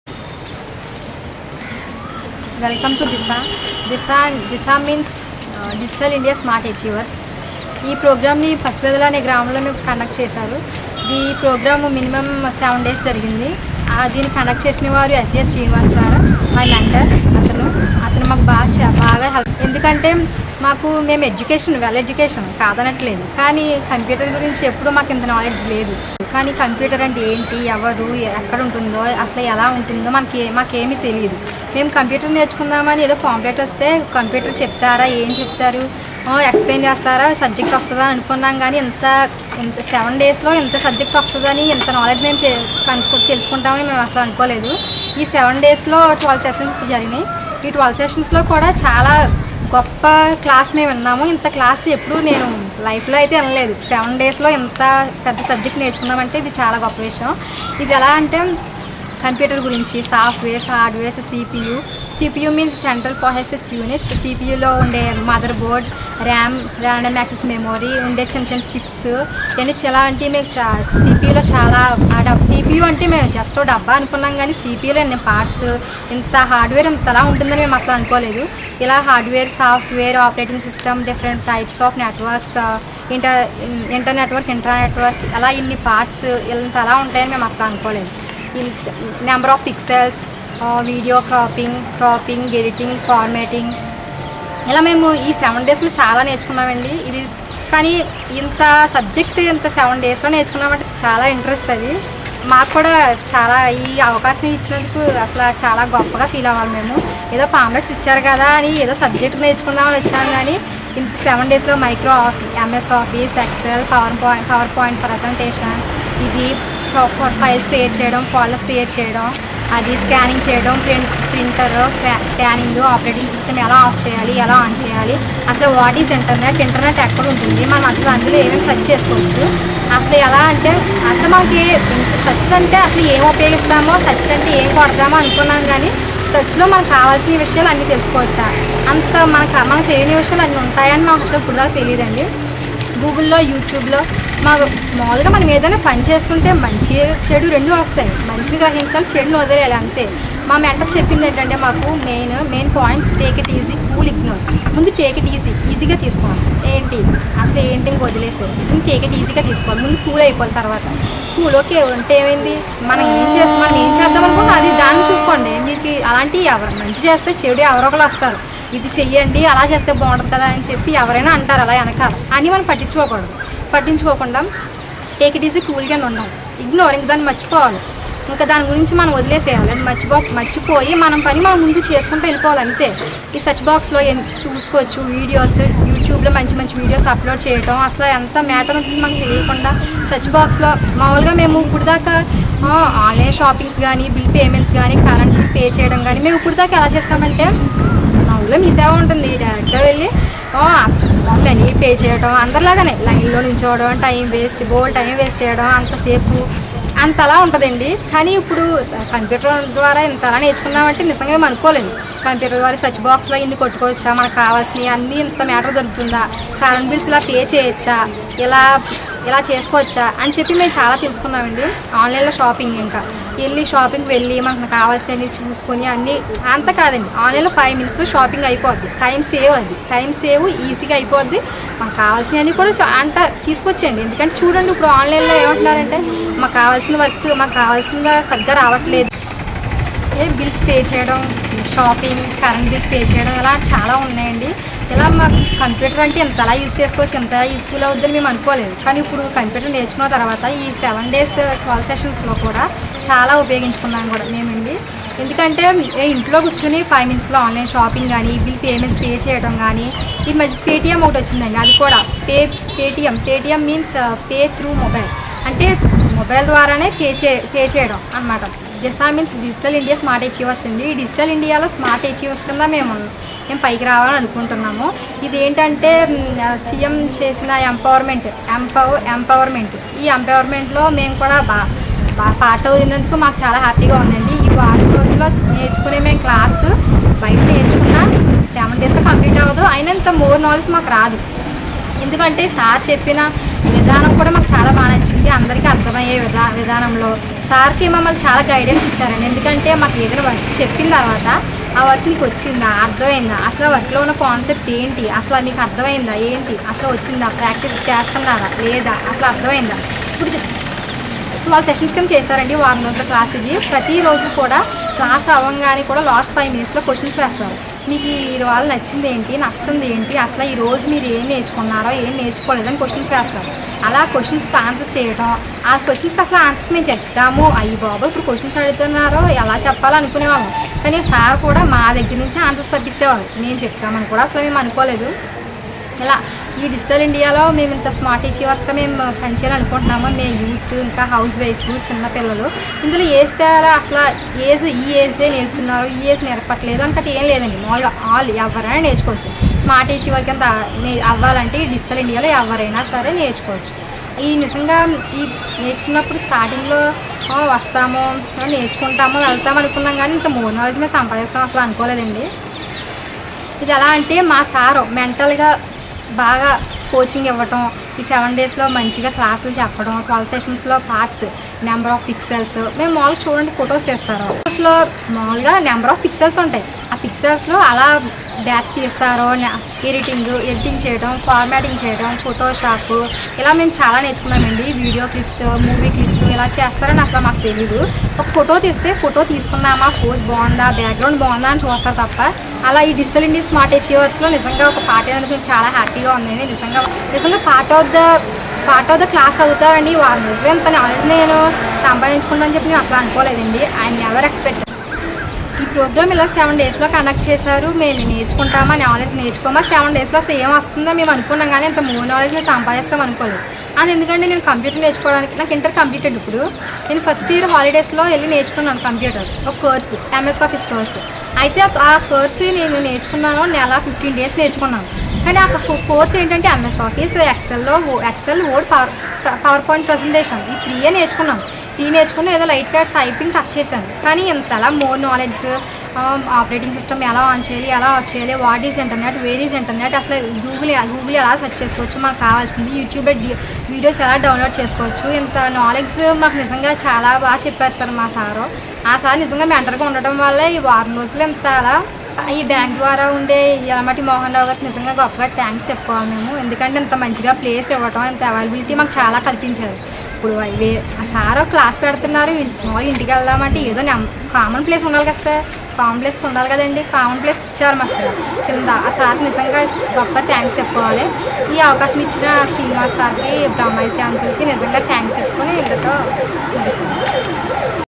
DISA PASIVEDALA YAMINI
Ms Yamini Vadapalli's feedback after she went through our unique computer training program "DISA" Towards Digital India Smart Achievers. DISA is a unique program we have developed for Rural Women Empowerment with a slogan "Let us delete the digital divide and Prove Digital India.
In this training we educate women so as to get good acquaintance with computer literacy and connect them to make use of the "Power of Internet". we are happy our first batch has itself becoming as trainers for the next batches. we are sure in every village women may have to be well empowered digitally so as to make use of all the benefits offered by dynamic governments.
We wish we will conduct such DISA training programs many more villages of West Godavari Dist soon, if any one looking for setting up our DISA programs may contact us at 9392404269